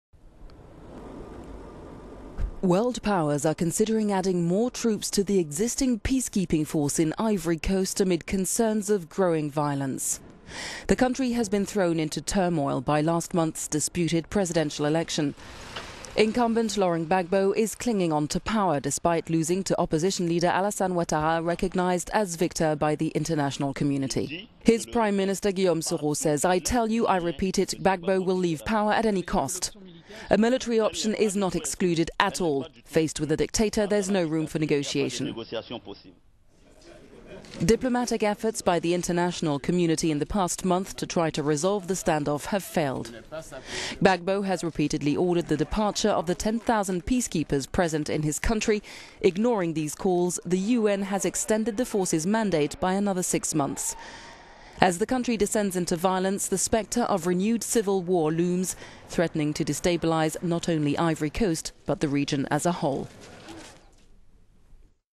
Ivory Coast: world powers discuss bolstering troops
World powers are considering adding more troops to the existing peacekeeping force in Ivory Coast amid concerns of growing violence.

The country has been thrown into turmoil by last month's disputed presidential election.

Incumbent Laurent Gbagbo is clinging on to power despite losing to opposition leader Allassance Ouattara, recognised as victor by the international community.